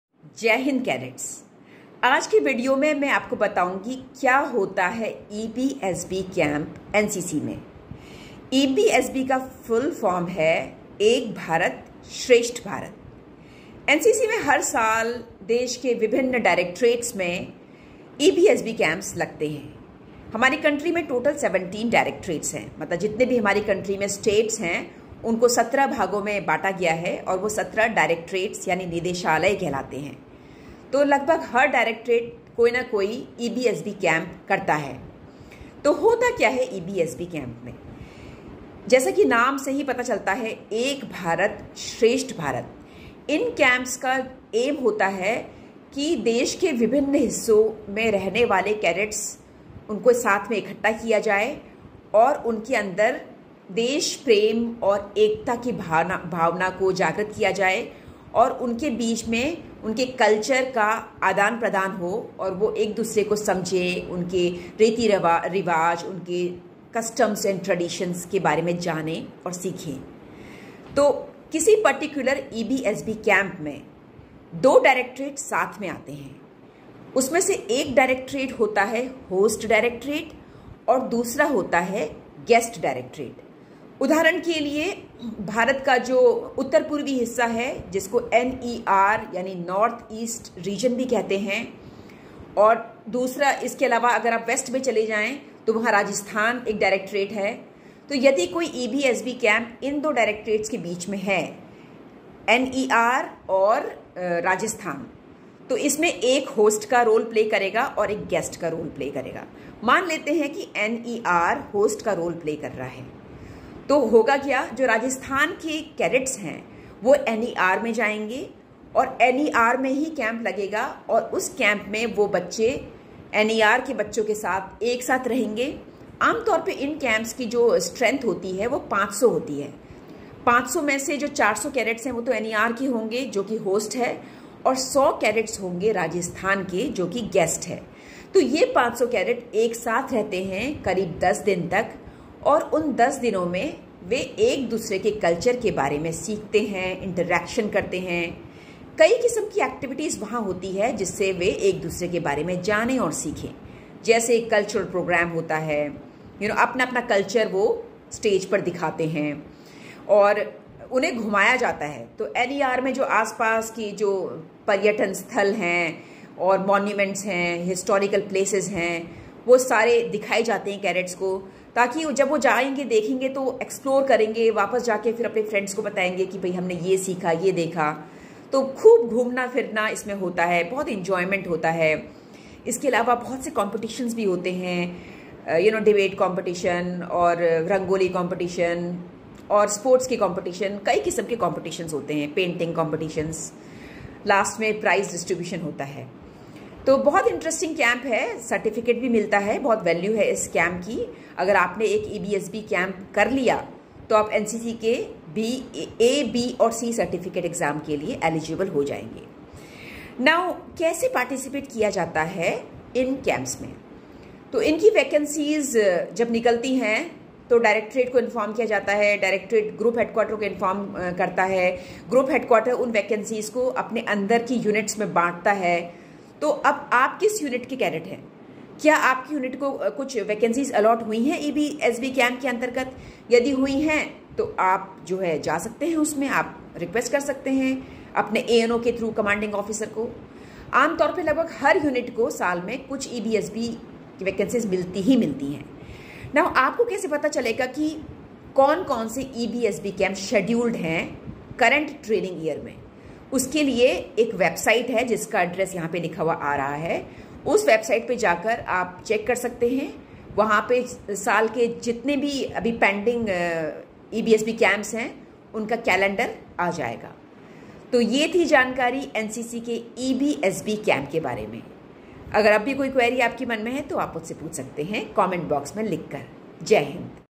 What is EBSB Camp in NCC ? | Ek Bharat Shreshtha Bharat
What is EBSB CAMP ?
How to participate in EBSB CAMP in NCC ?
When does the EBSB Camp happen?